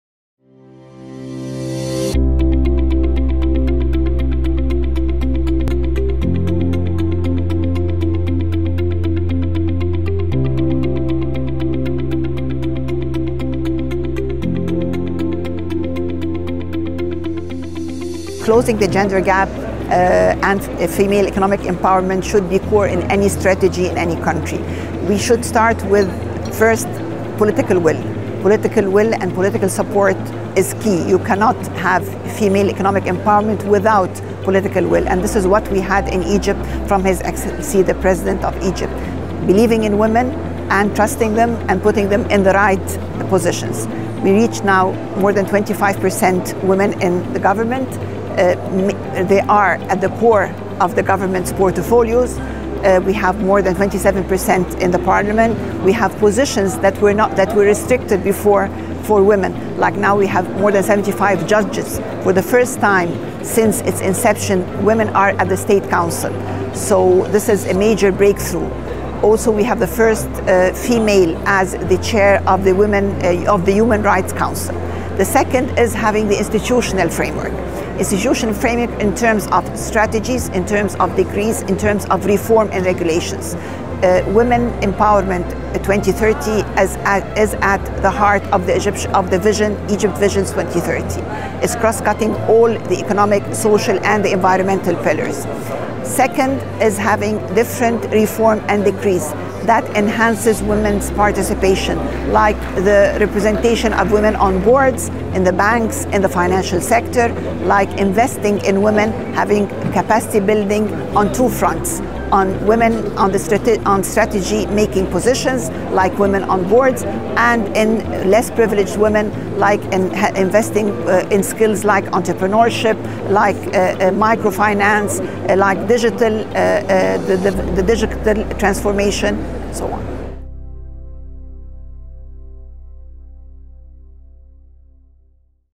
WGS - Hala El Said
At the World Government Summit, Dr. Hala El Said, Minister of Planning & Economic Development, Egypt, discussed with Forbes the main pillars of female economic empowerment.